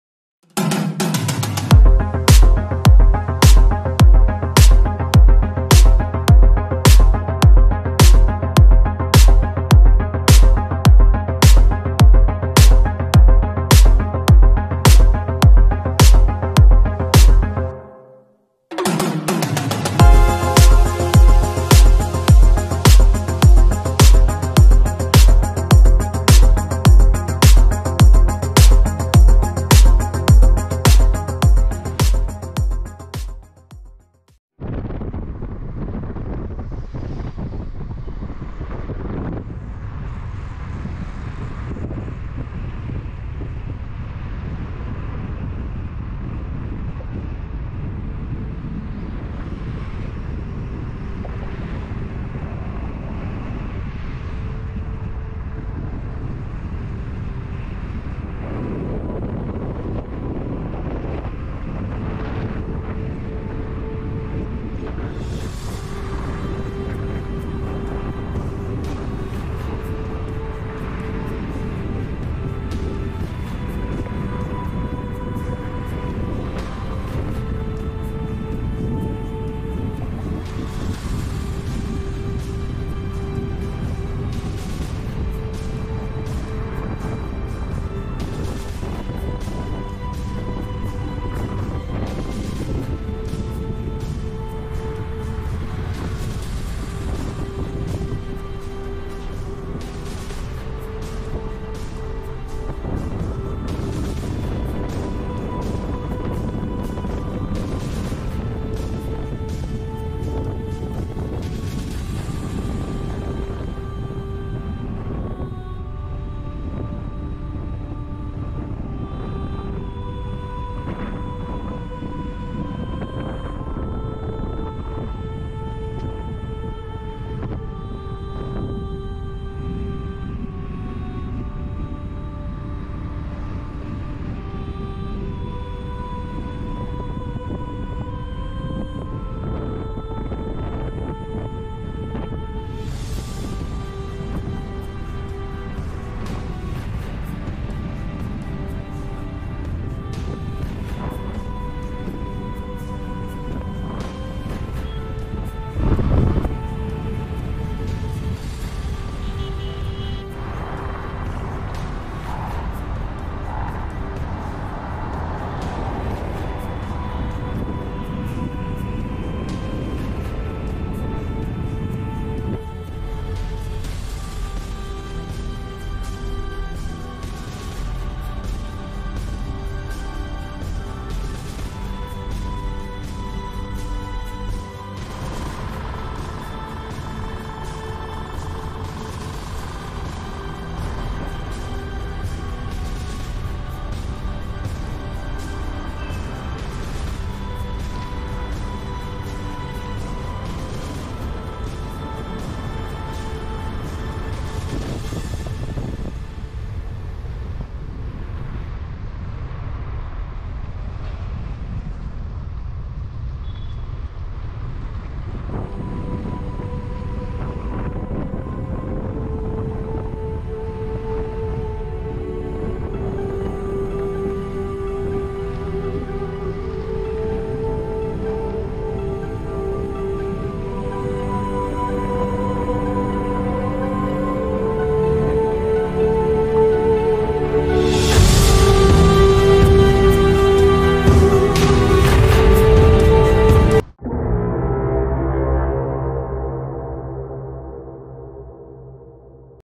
Hyderabad - New Hi-tech City | Tallest Buildings
Future Hyderabad - New Hi-tech City
- New Buildings in Hi-tech are so advanced and stylish.